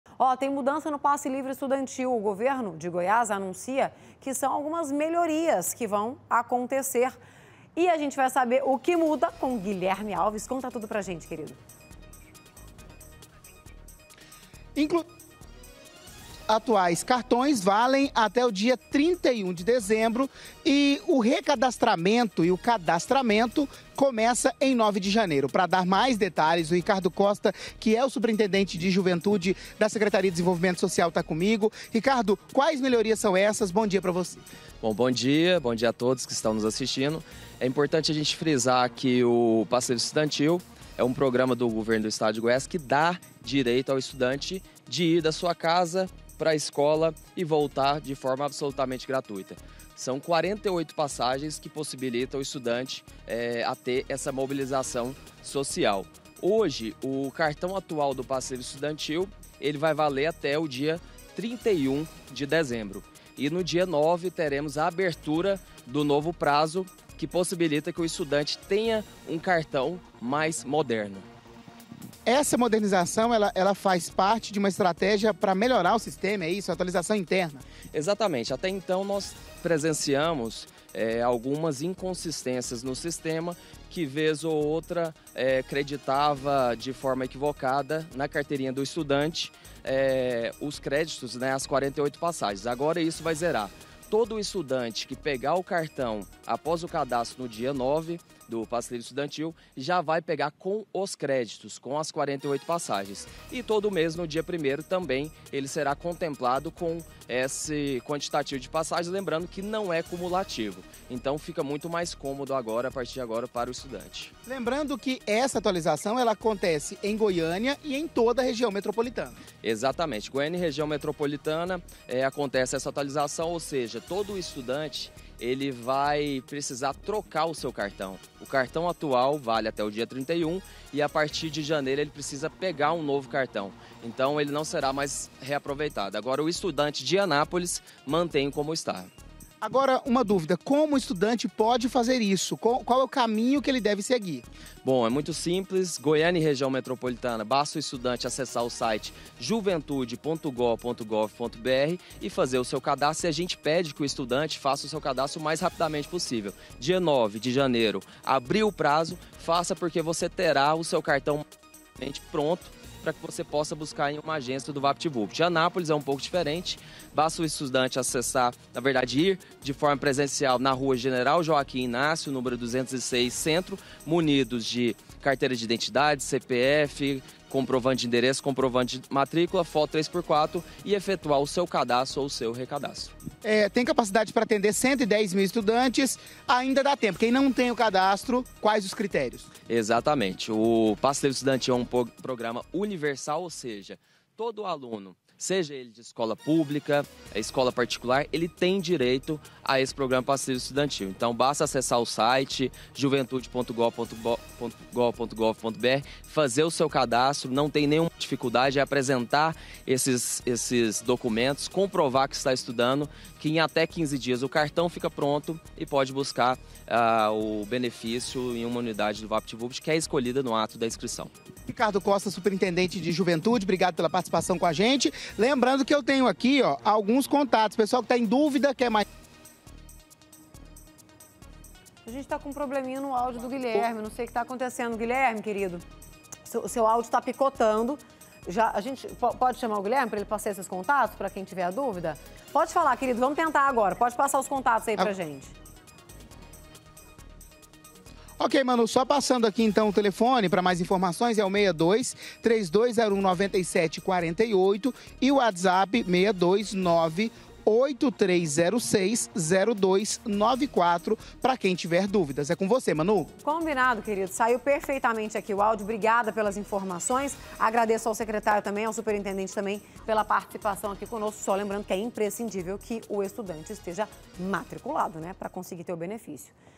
PASSE LIVRE ESTUDANTIL: GOVERNO ANUNCIA MUDANÇAS NO BENEFÍCIO
TEM MUDANÇA NO PASSE LIVRE ESTUDANTIL.O GOVERNO ANUNCIA QUE SÃO MELHORIAS. VAMOS SABER O QUE MUDA.